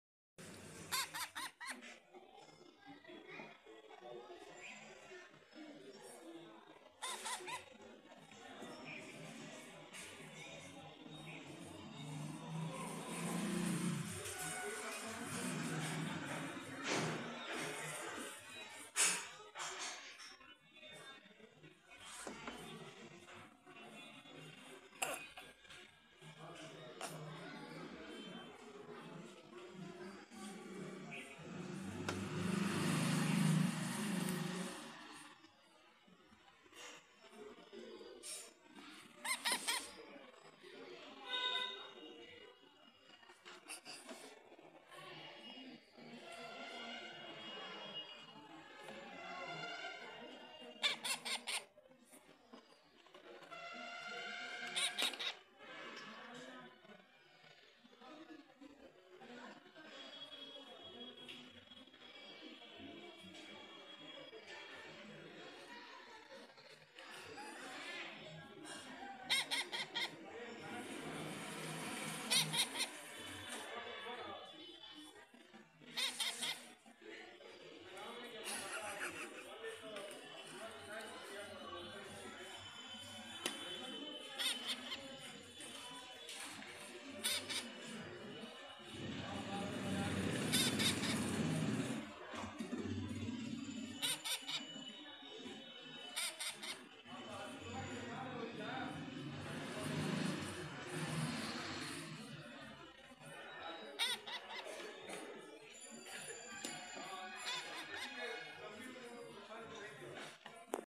Baby Parrot Eating Biscuits.. | Ringneck Parrot Eating Buscuit | Indian Ringneck Parrot
An Indian Ringneck Parrot Eating Biscuits. Ringneck Parrot Eating Buscuit.